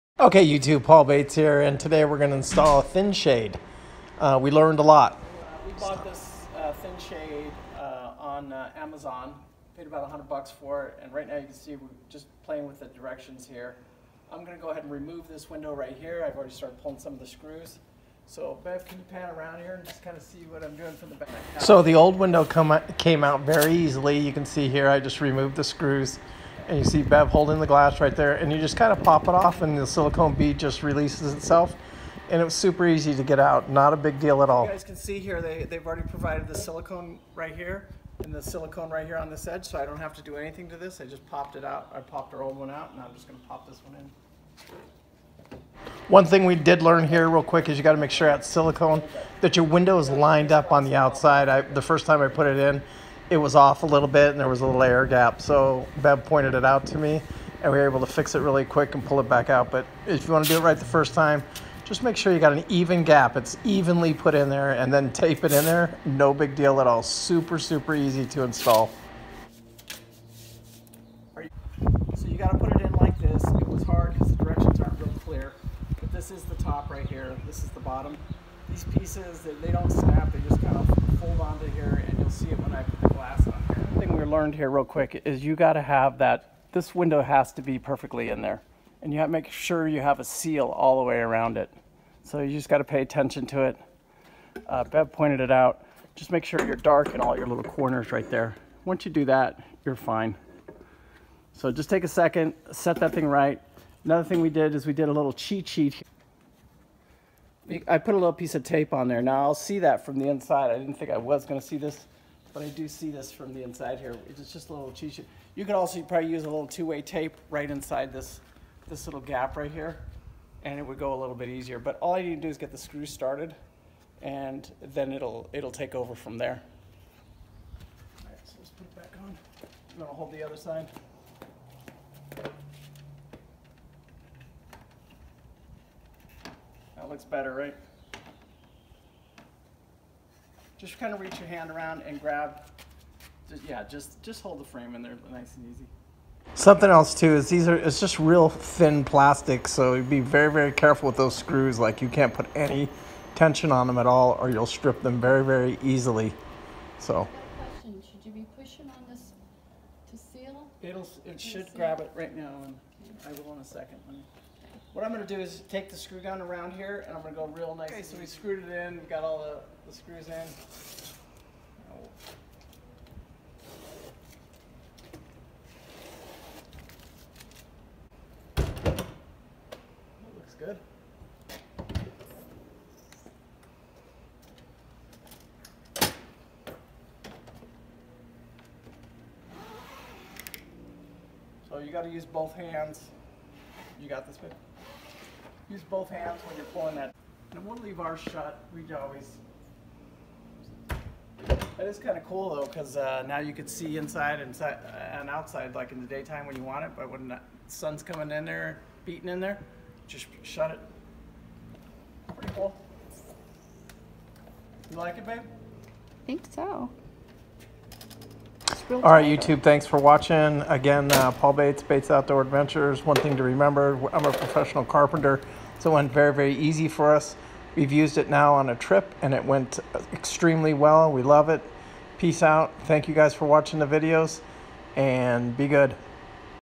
RV tips and tricks Entry Door Shade Install in our 2018 Forester Class C Entry door window
I installed a Thin shade in our entry door window. It was very easy for me to do. We have done many things to block the sun over the years in past RV's but this worked great.
Be very careful if using a screw gun. It does not take much to strip out the screws.
We bought ours on Amazon for less than 100.00